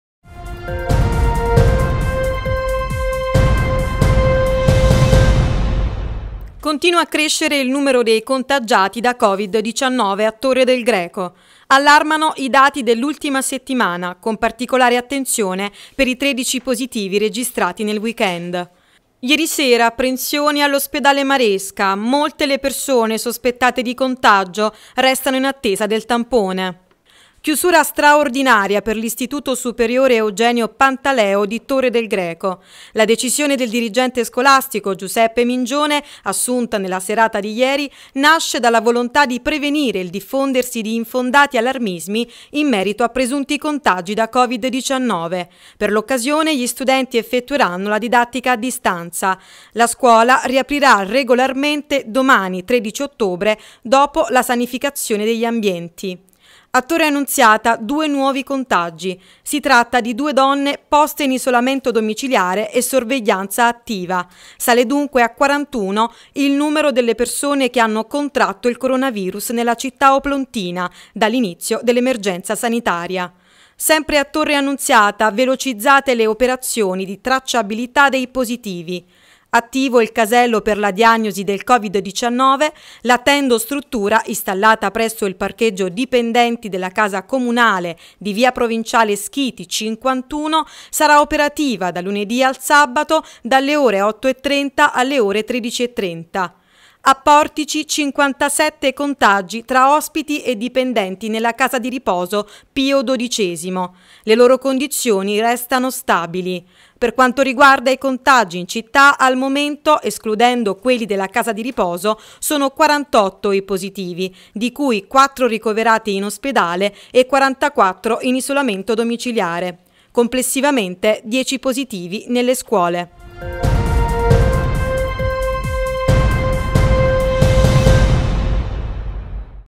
Aumentano i contagi nel Napoletano: il punto della situazione - VIDEO
Continua a crescere il numero dei contagiati da COVID-19 a Torre del Greco. Allarmano i dati dell’ultima settimana, con particolare attenzione per i 13 positivi registrati nel weekend.

Ieri sera apprensione all’ospedale Maresca. Molte le persone sospettate di contagio, restano in attesa del tampone.

Chiusura straordinaria per l’istituto superiore Eugenio Pantaleo di Torre del Greco. La decisione del Dirigente scolastico, Giuseppe Mingione, assunta nella serata di ieri, nasce dalla volontà di prevenire il diffondersi di infondati allarmismi in merito a presunti contagi da COVID-19. Per l’occasione gli studenti effettueranno la didattica a distanza. La scuola riaprirà regolarmente domani, 13 ottobre, dopo la sanificazione degli ambienti.

A Torre Annunziata due nuovi contagi: si tratta di due donne poste in isolamento domiciliare e sorveglianza attiva. Sale, dunque, a 41 il numero delle persone che hanno contratto il Coronavirus nella città oplontina dall’inizio dell’emergenza sanitaria.

Sempre a Torre Annunziata velocizzate le operazione di tracciabilità dei positivi. Attivo il casello per la diagnosi del COVID-19 La tendostruttura, installata presso il parcheggio dipendenti della Casa Comunale di via Provinciale Schiti, 51, sarà operativa dal lunedì al sabato, dalle ore 8,30 alle ore 13,30.

A Portici 57 contagi tra ospiti e dipendenti nella casa di riposo Pio XII: le loro condizioni restano stabili. Per quanto riguarda i contagi in città al momento, escludendo quelli della casa di riposo, sono 48 i positivi, di cui 4 ricoverati in ospedale e 44 in isolamento domiciliare. Complessivamente 10 i positivi nelle scuole